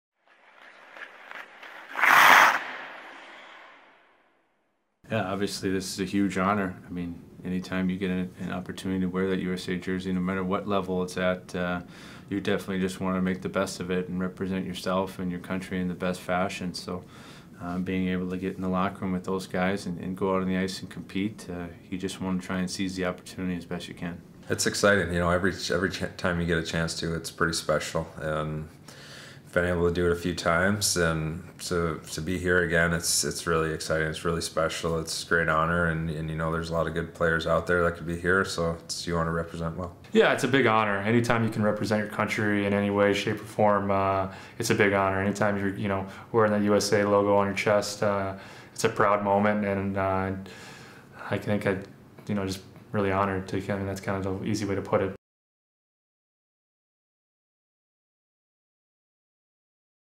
2016 World Cup of Hockey: What it Means to Represent Team USA
For members of Team USA at the World Cup of Hockey, representing their country is something they all share a great appreciation for. For more coverage of the 2016 World Cup of Hockey visit teamusa.usahockey.com